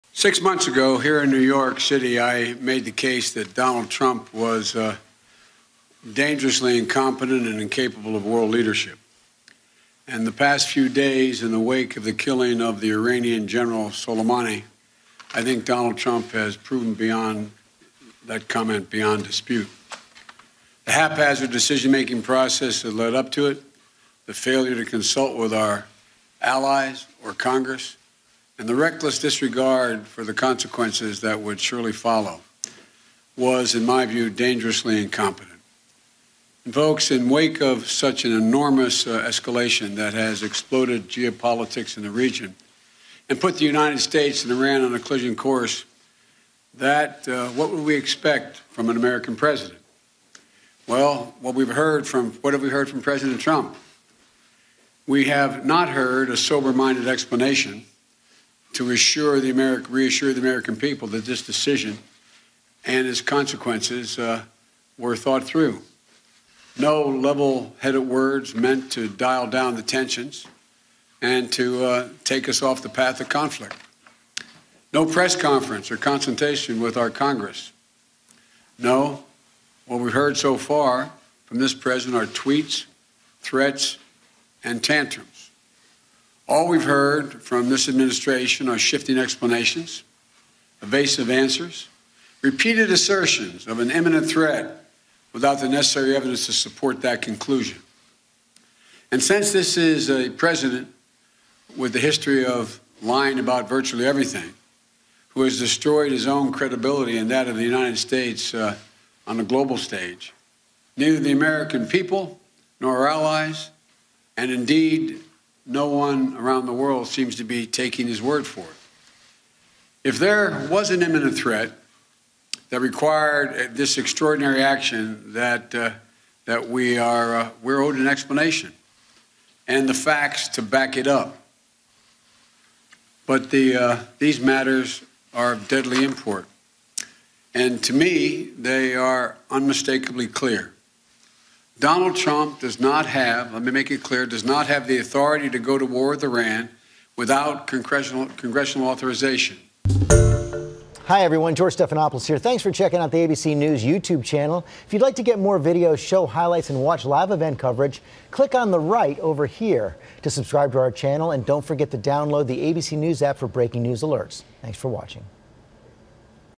Trump does not have authority to start war with Iran: Biden l ABC News
Presidential candidate Joe Biden discussed tensions with Iran and the U.S. airstrike that killed Gen. Qassem Soleimani.